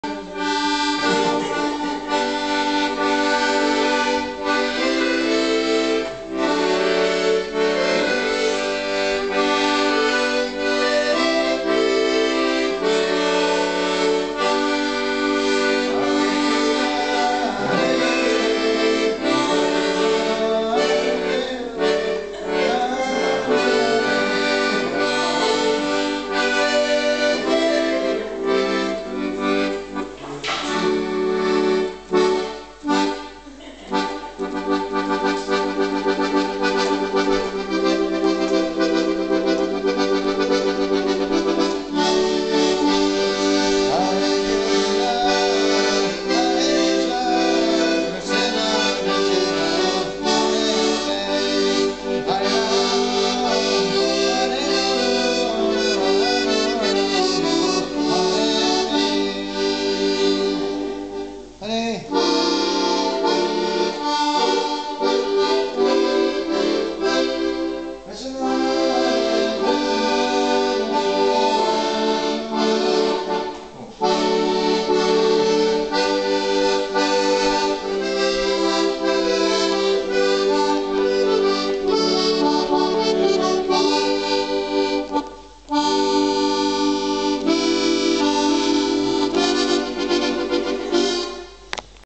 Sukhi on the squeezebox
Sukhi Krishna dasa practicing tunes for harinama at Govinda's, Swansea.